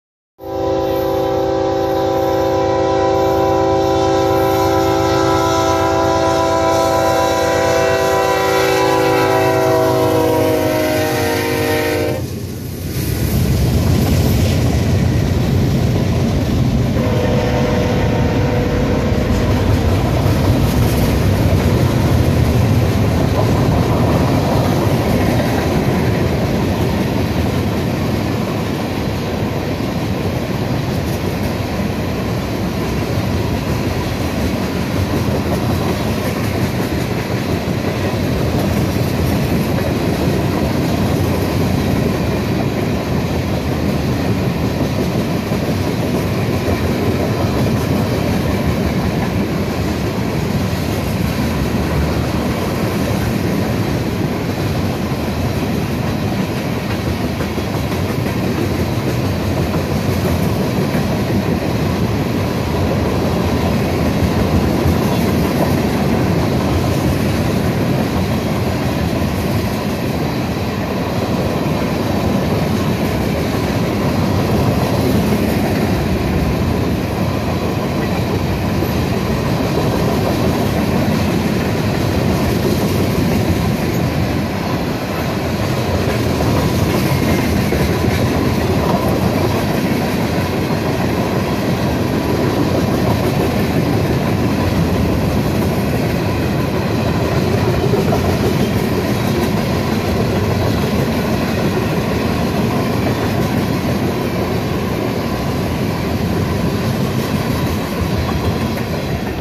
BNSF y Santa Fe en México 8087, 665 ES44C4, C44-9W en Lagos de Moreno, Jalisco. México NOAS_5
Buenas raza aquí les dejo un vídeo de un tren de Ferromex Rumbo al Norte que en esta ocasión traía 1 locomotora de la BNSF y una Santa Fe, la líder fue una ES44C4 la 8087 BNSF y en múltiple venía una C44-9W la 665 Santa Fe, este par de BNSF a su paso por México lo grabe en la placa Kilométrica # 468 de la Línea A del Distrito de León en el municipio de Lagos de Moreno, Jalisco. México Saludos Gente NOAS_5
NOAStudioPhotos
FerroNoas